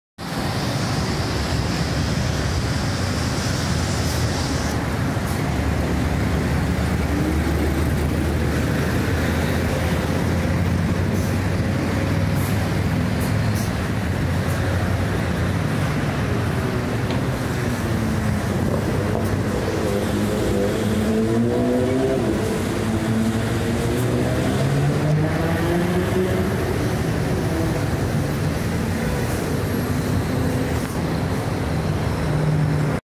Terminal metrô Tatuapé SP 09
Primeiro carro
4 5346
Caio apache VIP IV
VW 15-190OD Euro V
Allibus transportes
Segundo carro
4 5656
Neobus spectrum city
VW 15-190OD
Allibus transportes
Filmado no terminal metrô Tatuapé